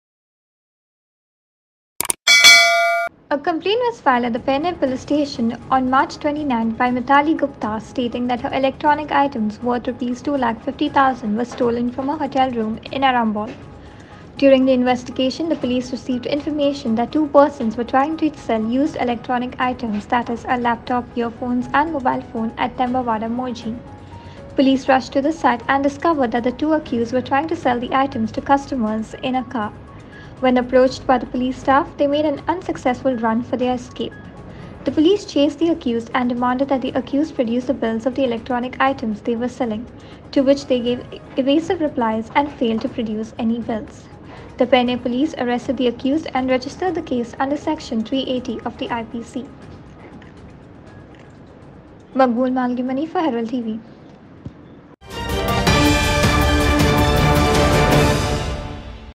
Two shops gutted in fire at Tivim railway station
Two shops located at the Tivim Railway Station, were gutted in fire on Friday Morning. The fire broke at around 4 am. Fire personnel immediately rushed to the site and began the fire fighting operations. The cause of the fire is yet to be known. Both the shop owners suffered a combined loss of around Rs 8lakh